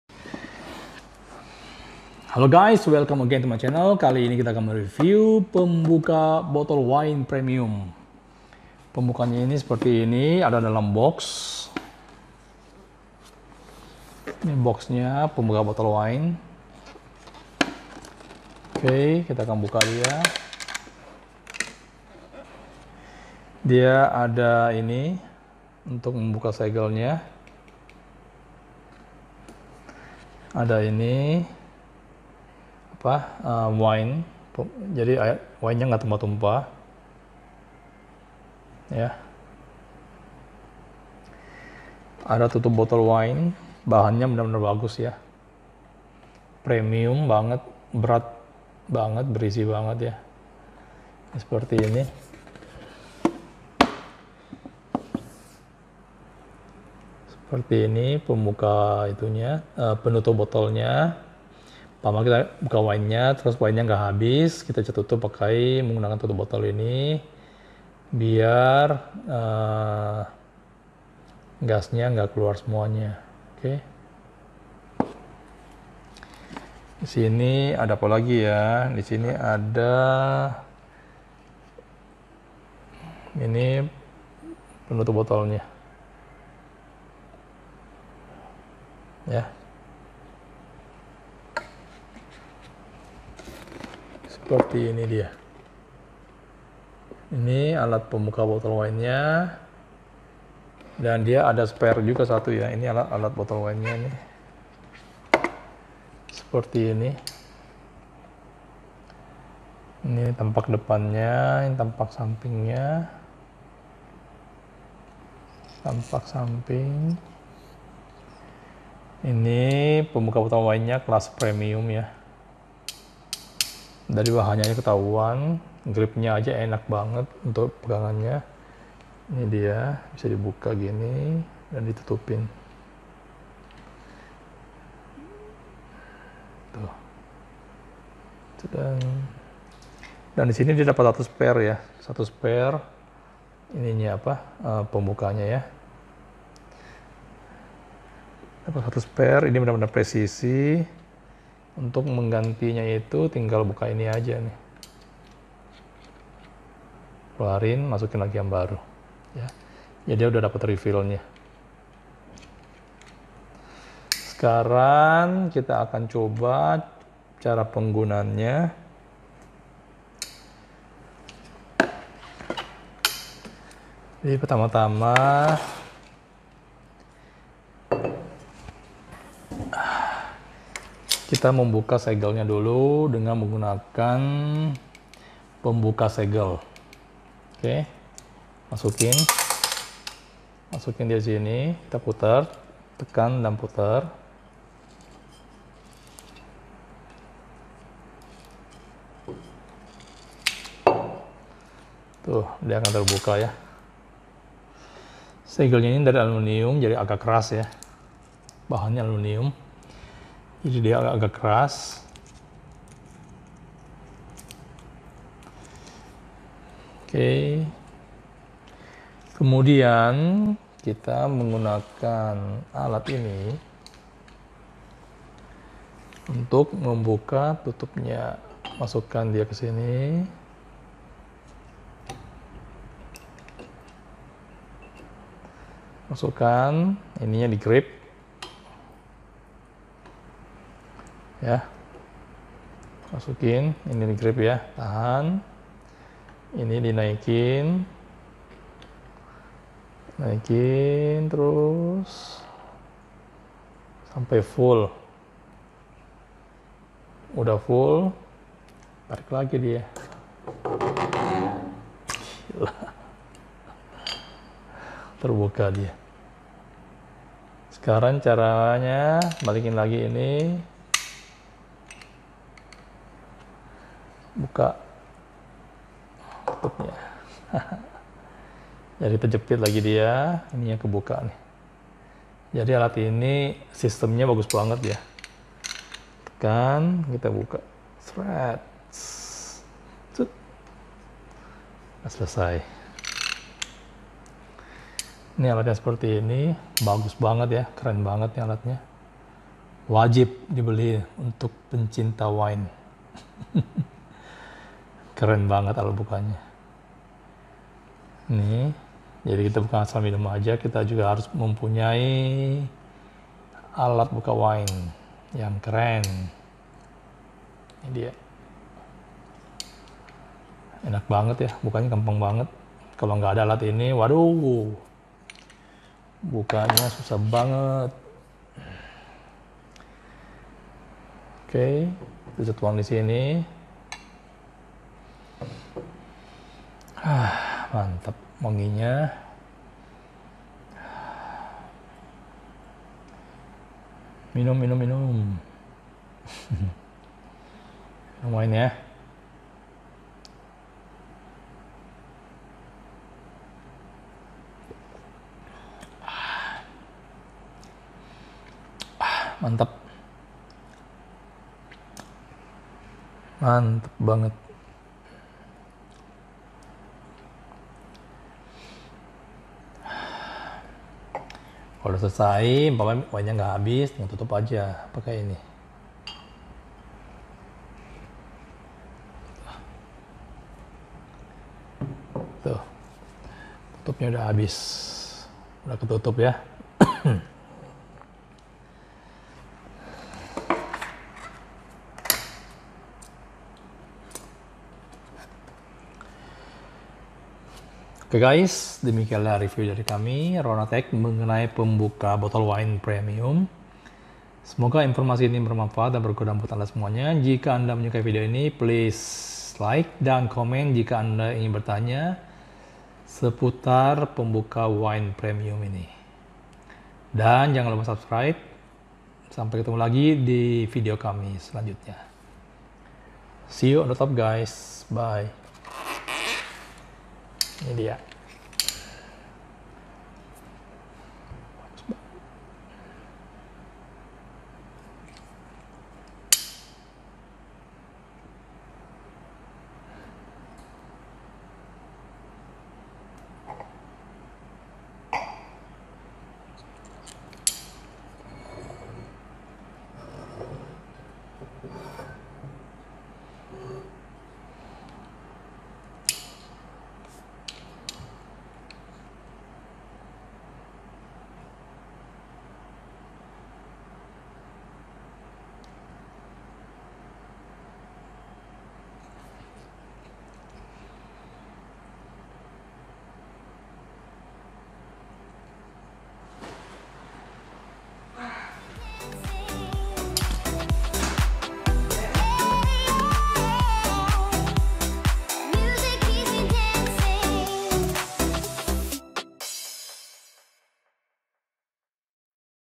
Review Pembuka Tutup Botol Wine Premium Seharga Rp. 5.000.000
pembuka botol wine